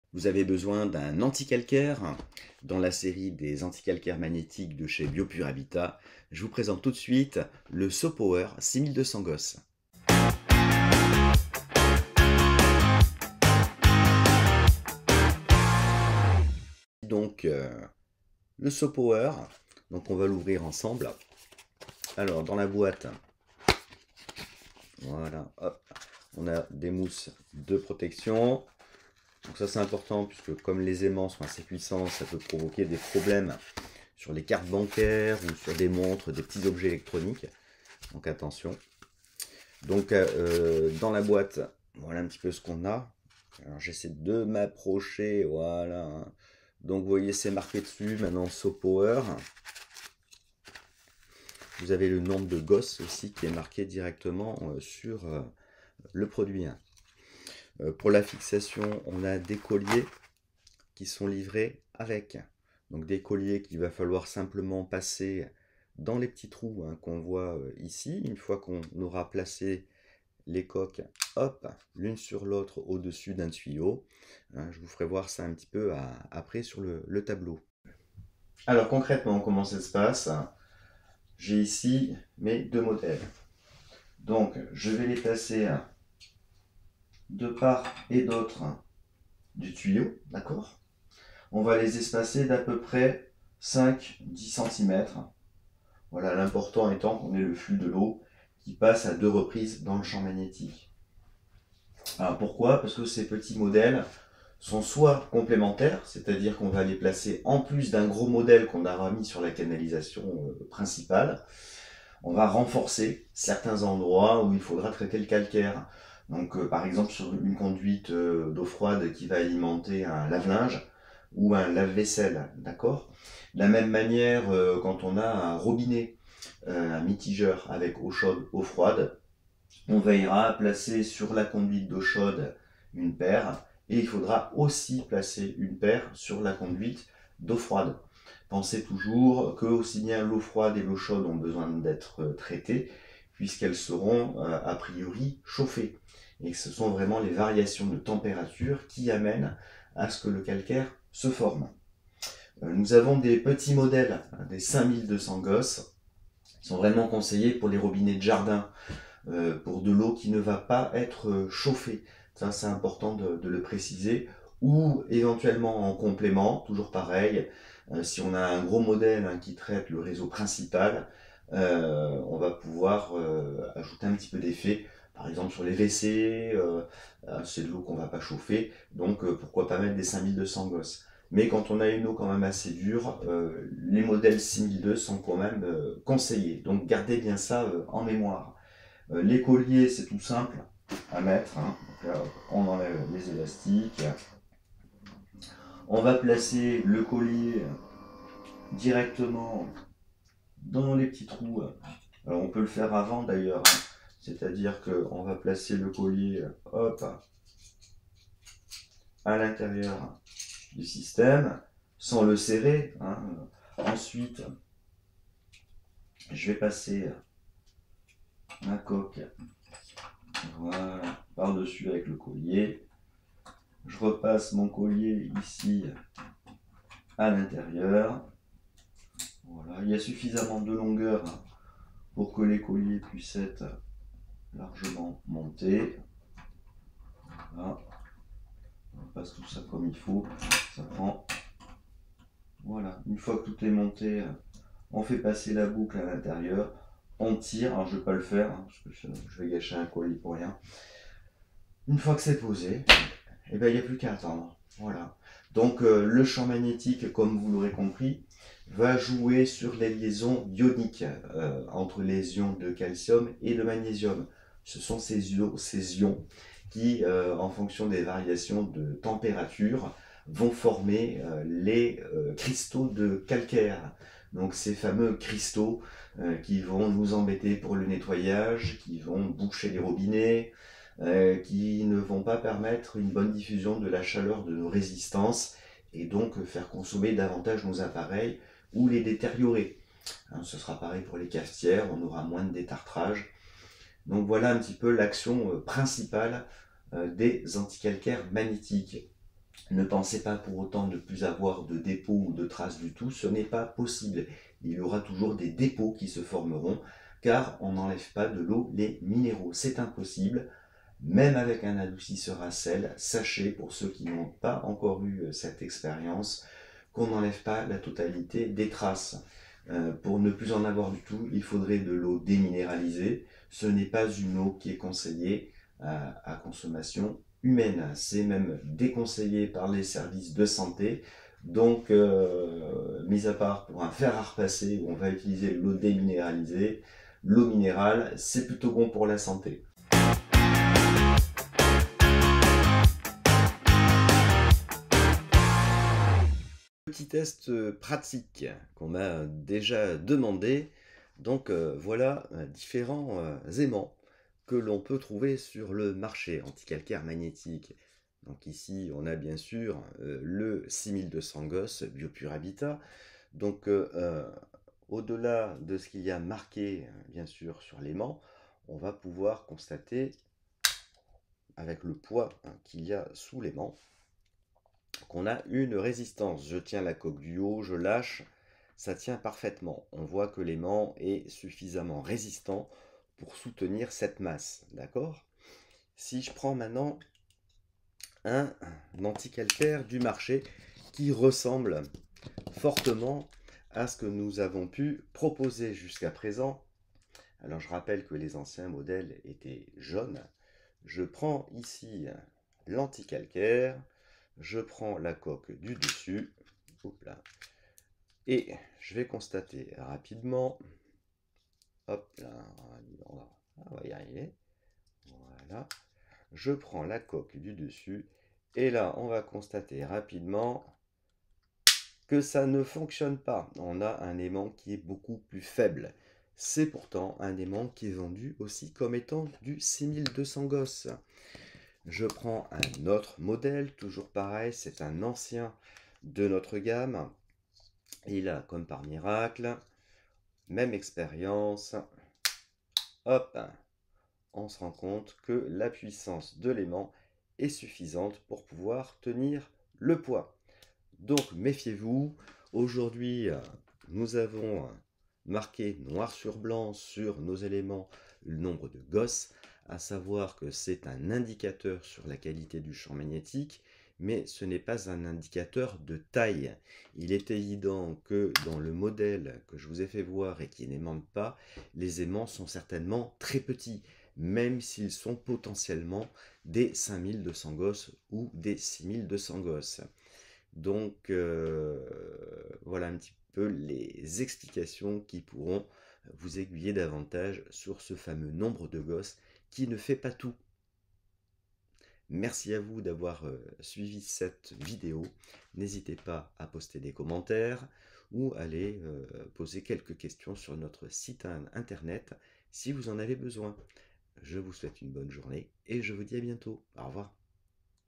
Aimant anti calcaire magnétique So Power 6200 gauss
Le calcaire dans la maison, c'est du nettoyage et de l'entretien dont on aimerait bien se passer. Les anti calcaires magnétiques sont une solution économique et efficace pour limiter l'apparition de ce tartre. Alors que certains adoucisseurs à sel vont retirer une partie des minéraux, les anti calcaires magnétiques vont modifier la cristallisation des minéraux.
Attention à bien choisir le modèle adapté...